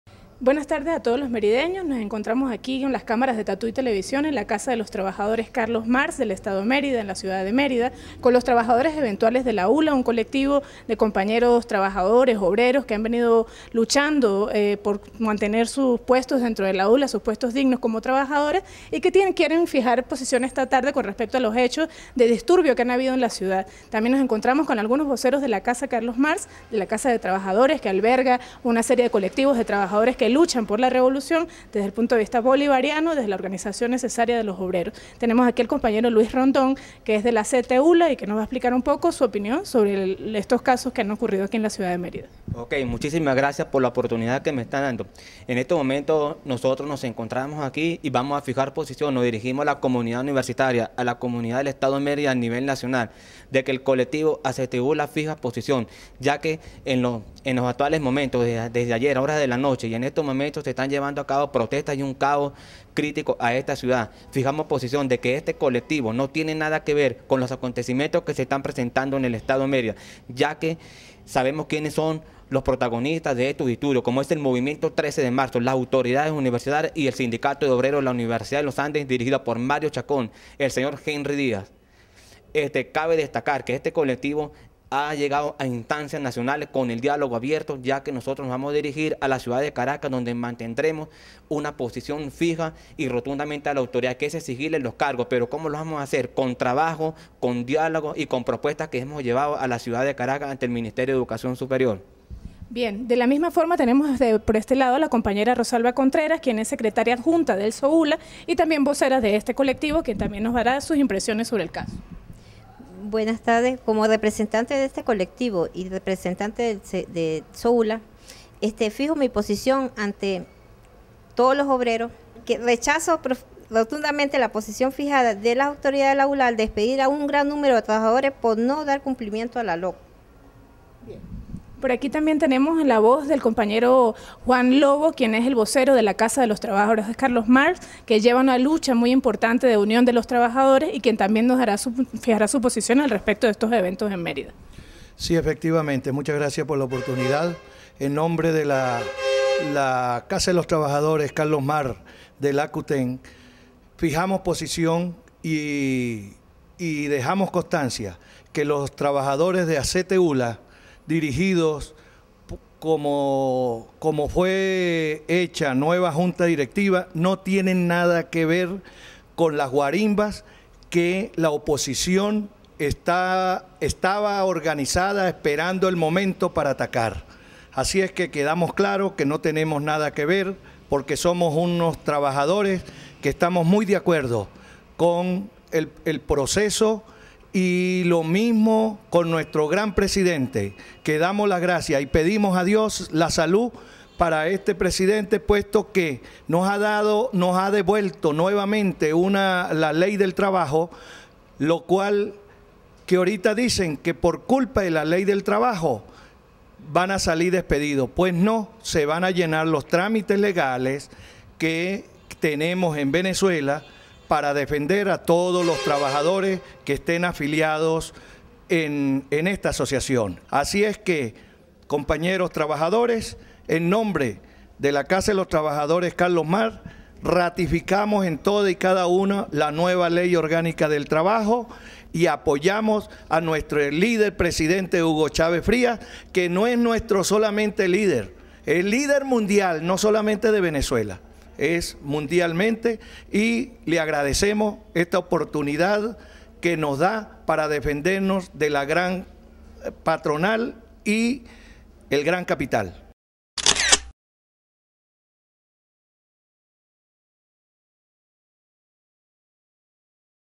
Aquí Tatuy Informa: ACTEULA fija posición ante disturbios en la ciudad de Mérida
En Mérida en la última semana, ha habido un gran movimiento de protestas y manifestaciones en contra de la intención de despidos injustificados que el rector de la ULA ha querido imponer sobre los trabajadores eventuales y contratados; despojando a cientos de hombres y mujeres de sus trabajos dignos, y negándose a cumplir con la nueva LOT, aprobada por el Comandante Chávez, este mes de mayo. Varias organizaciones de trabajadores y obreros han salido a manifestar de manera vioelenta en la ciudad, pero otros colectivos como el ACTEULA, piden a sus compañeros recapacitar y adelantar la lucha necesaria por sus cargos fijos, pero de manera pacífica, a través del diálogo y apoyándose en el MInisterio del Poder Popular para la Educación.